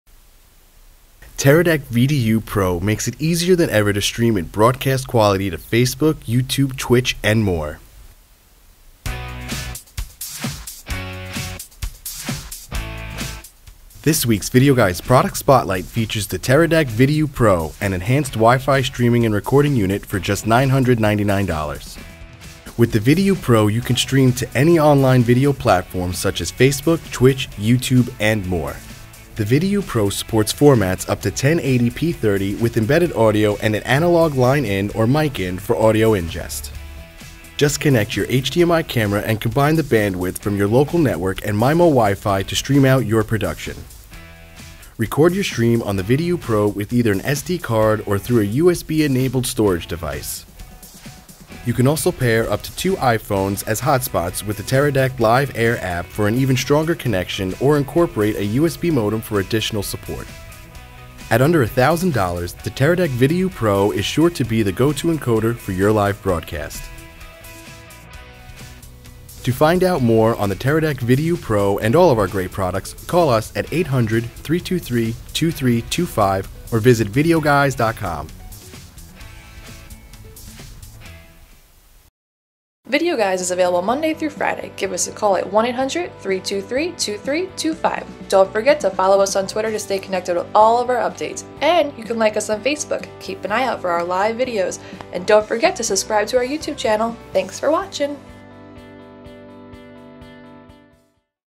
Teradek VidiU Pro Videoguys Product Spotlight
This week's Videoguys Product Spotlight is on the Teradek VidiU Pro enhanced WiFi streaming and recording unit.  This compact video encoder allows you to stream to any platform with a local ethernet connection and their MIMO WiFi technology.  With the Teradek VidiU Pro you can also record with either an SD card or an external hard drive via it's USB connection.  At under $1,000 the VidiU Pro video encoder is just the device you need to get your production up and running.